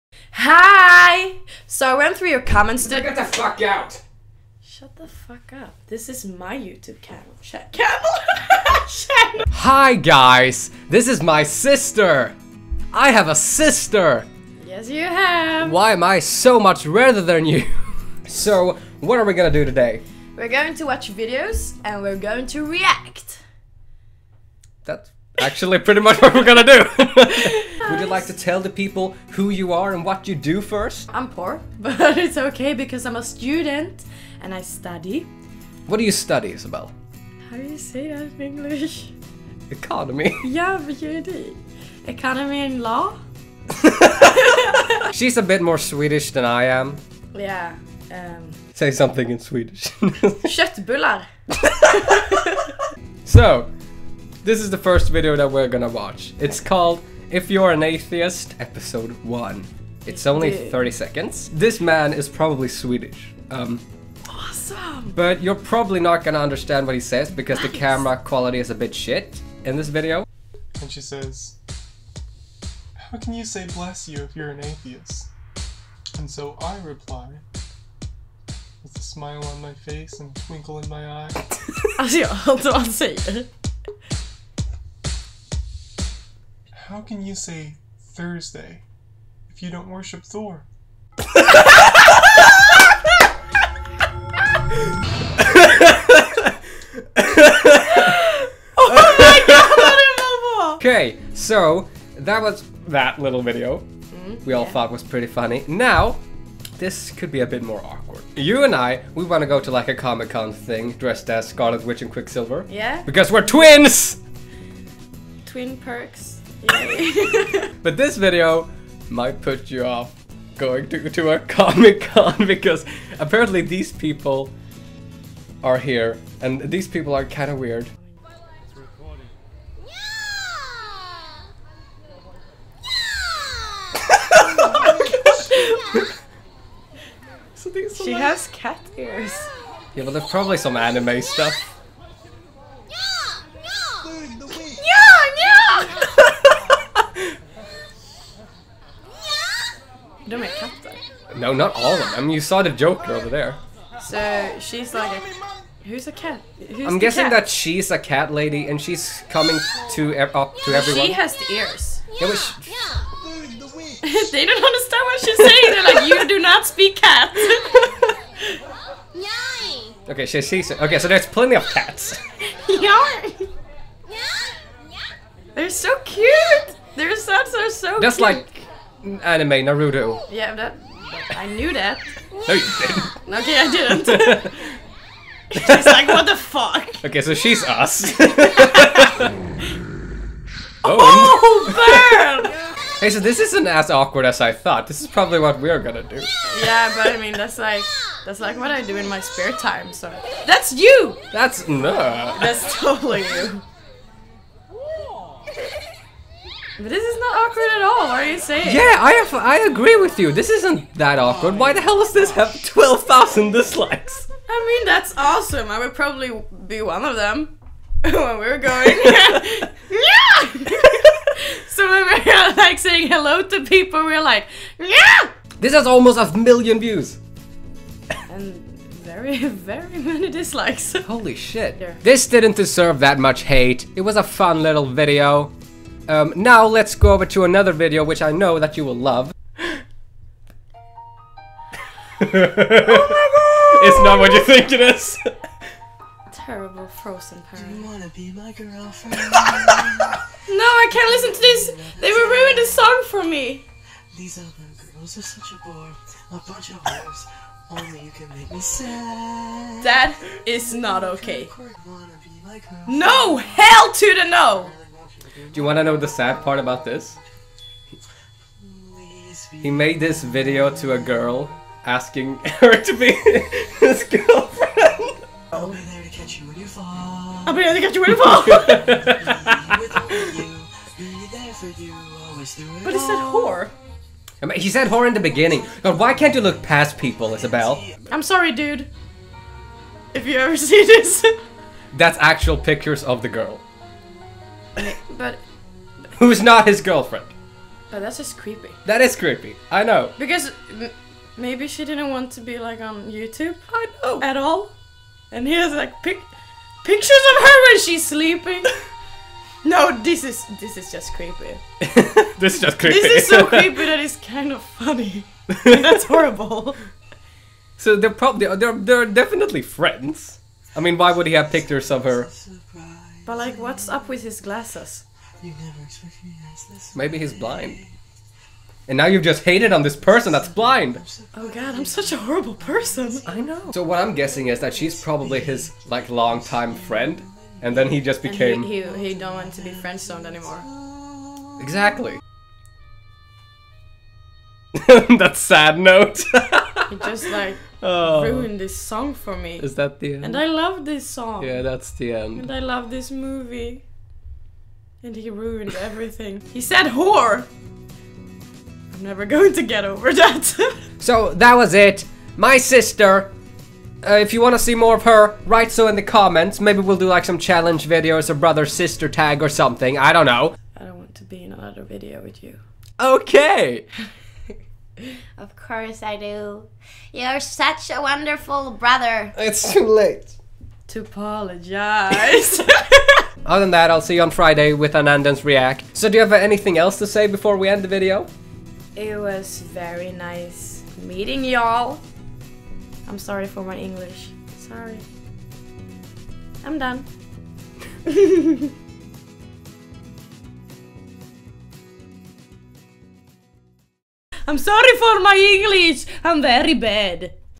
Frozen Parody & Cat convention (Anden&Sister)
This is not an Anden Reacts, which is why this isn't uploaded on a Friday. This is just a fun little video that I decided to make with my sister. I've picked out three video for us to react to. First a short video where a dude debates religion, the second video is of a comic convention, and the last is a parody of